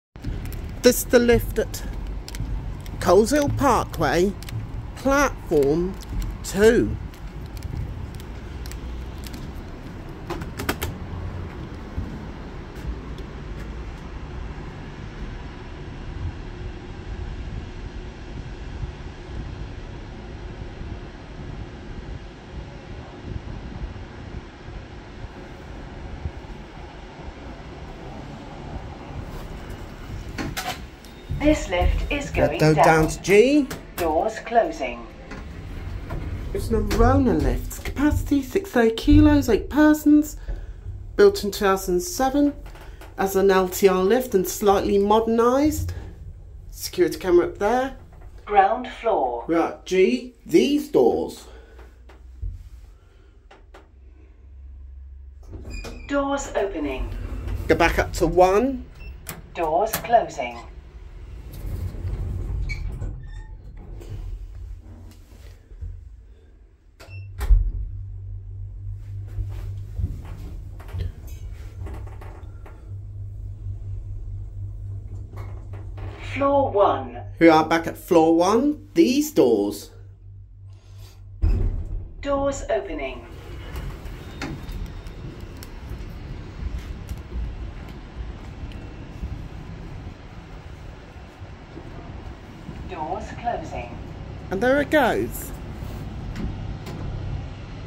2007 LTR (Slightly Modernised by Orona) Lift @ Coleshill Parkway Platform 2
The Lift has a Separate Entrance and Exit Door